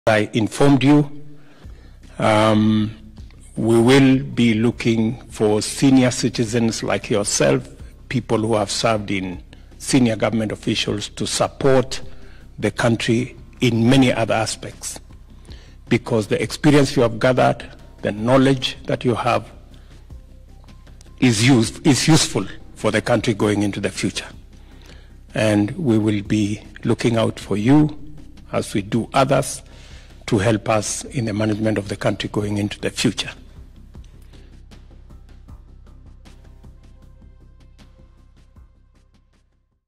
Ruto: We’ll look for retired senior citizens to help manage govt institutions
President William Ruto has said that 'the government will look for senior retired citizens to help in the management of the country.